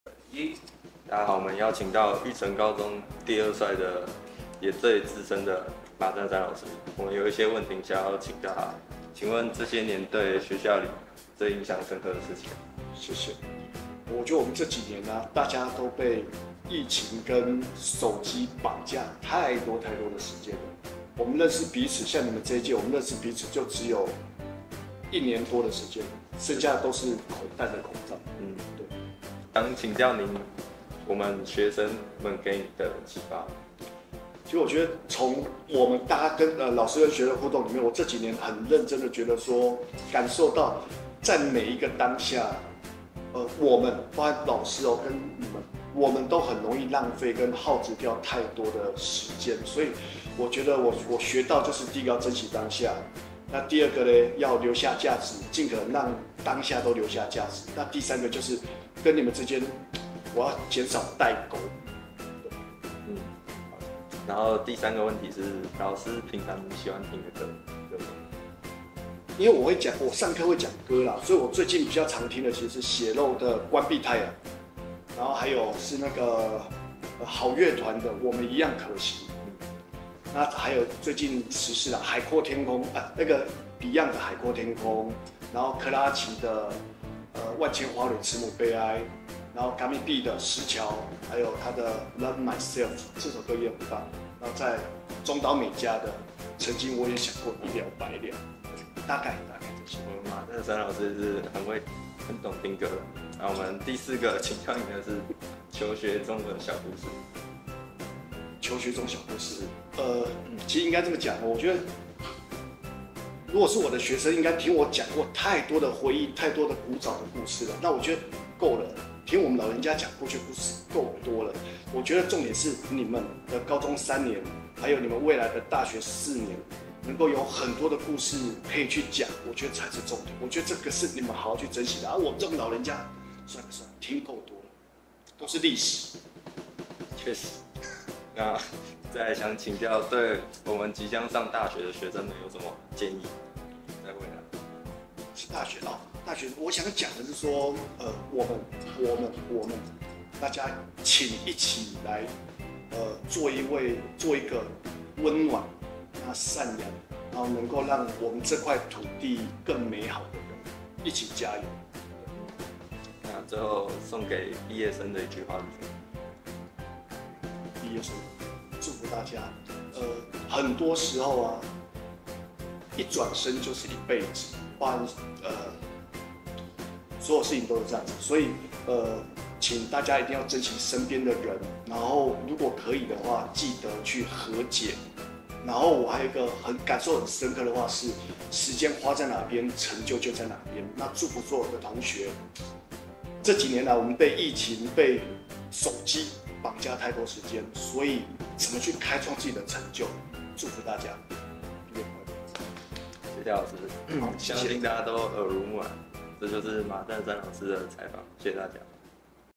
【一轉身就是一輩子】 不可能吧~畢業週系列採訪 -3
育成高中第二十屆畢業典禮在即，推出畢業週系列活動，
包含採訪影片、許願牆、倒數等多項活動，
本集邀請到育成第二帥的老師(喂 ~  來和畢業生們聊聊天，

本集金句:做一個溫暖、善良，可以讓這塊土地更美好的人

畢業典禮將於2024年06月04日太陽時間8:50於育成高中6樓體育館舉行，
不可能不來吧！

00:09-00:30 【這些年校園內印象深刻的事】
00:30-01:04 【教學相長--學生所引發的人生體悟】
01:04-01:43 【還是要聽歌吧~老師的推薦歌單】
01:43- 02:19【求學過程中的小趣事-創造自己的故事】
02:19- 03:30【給畢業生的人生小tips】